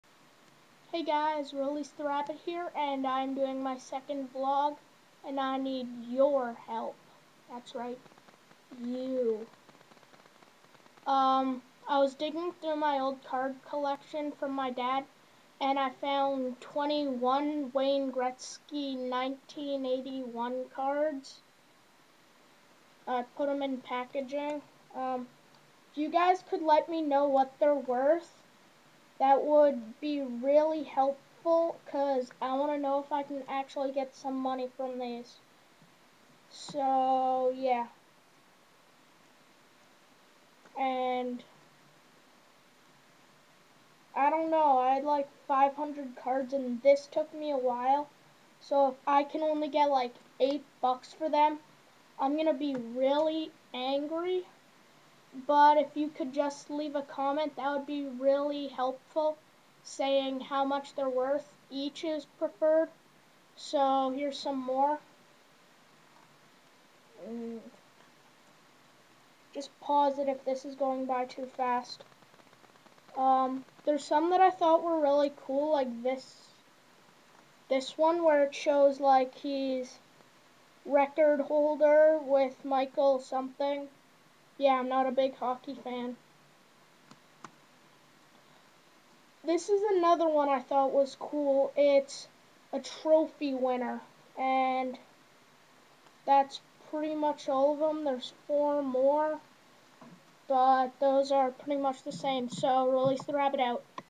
releasetherabbit 2nd vlog wayne gretzky cards and prices
my second vlog talking aboult wayne gretsky cards i found